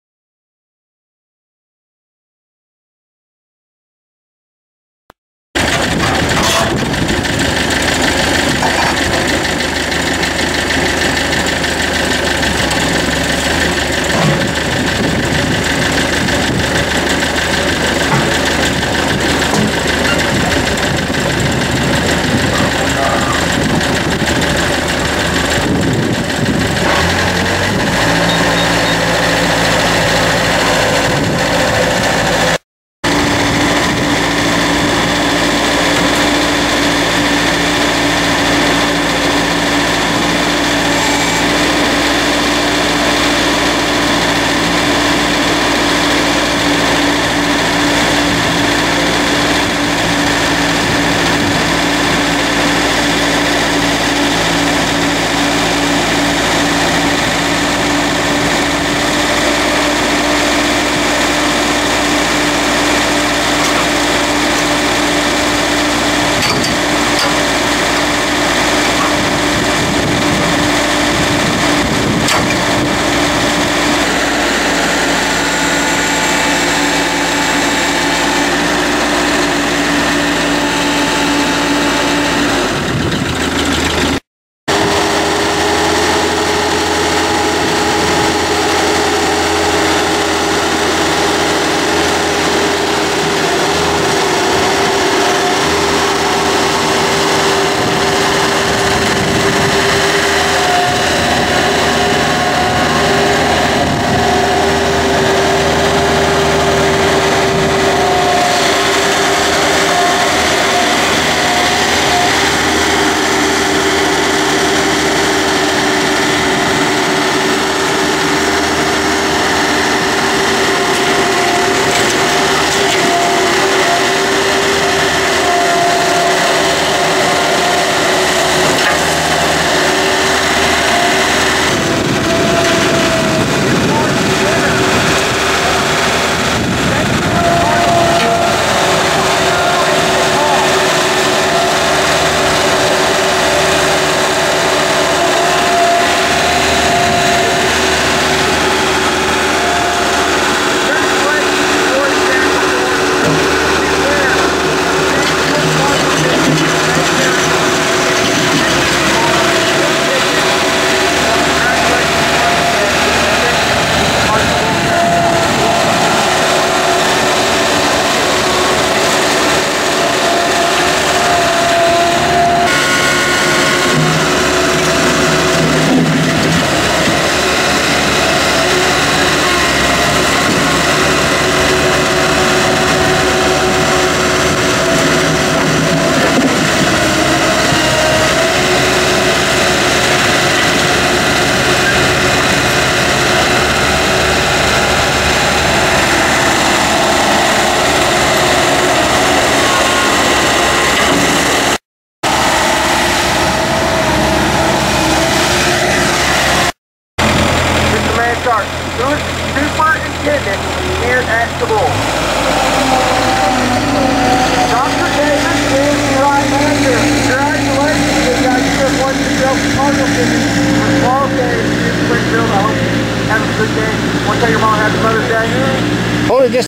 DIGGER DEMO MOVIE
Introducing the BISHOP ROUGHNECK a new versatile piece of equipment that can be attached to almost any tractor or skid steer.